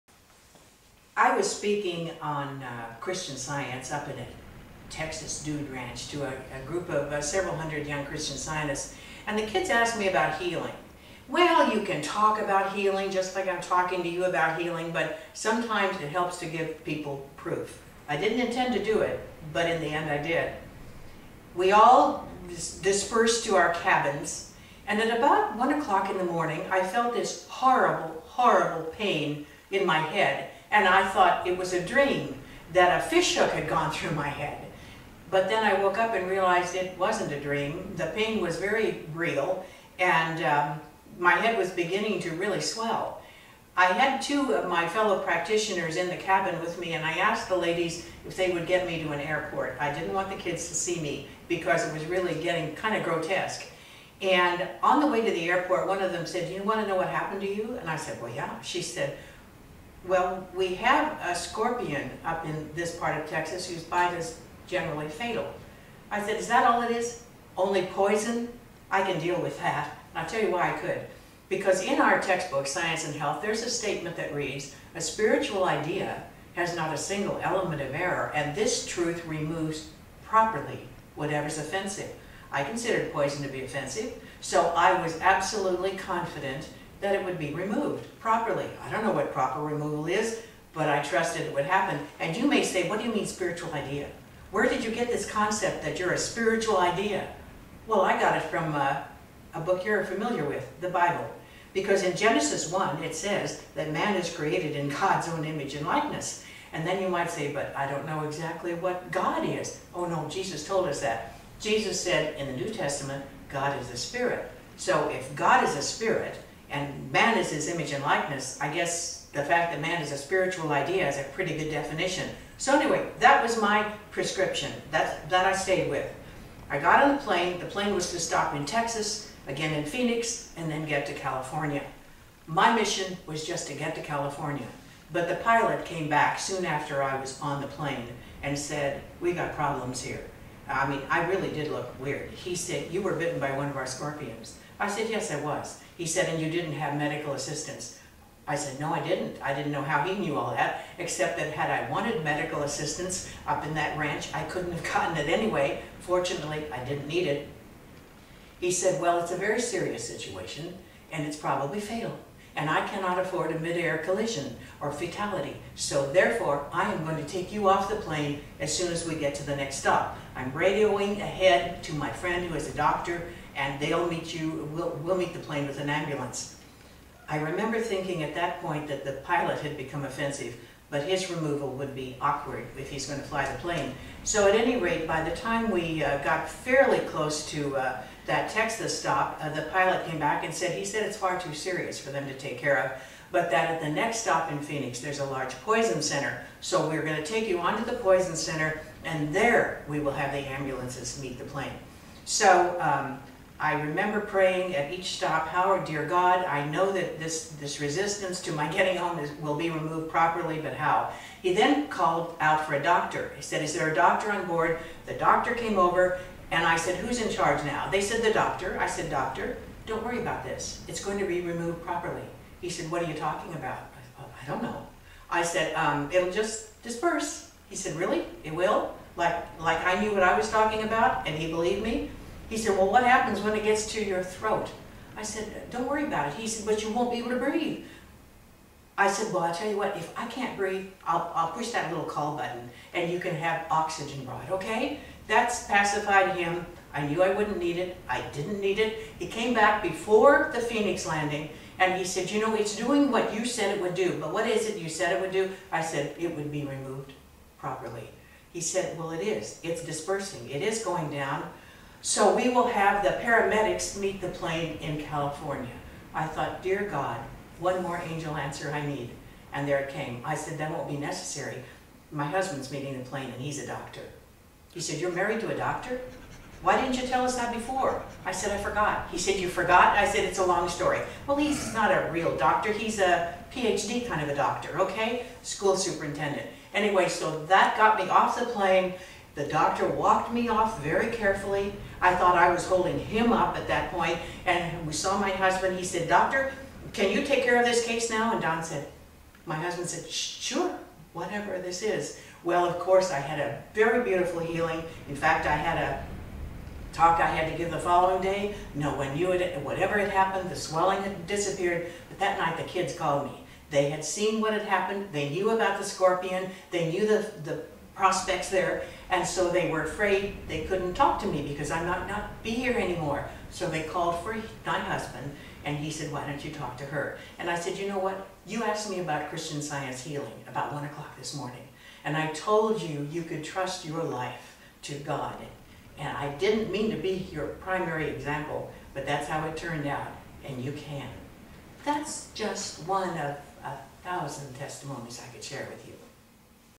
Deadly Scorpion Sting Healed
I was at a Texas dude ranch and felt a horrible pain in my head... This is a testimony of Christian Science healing.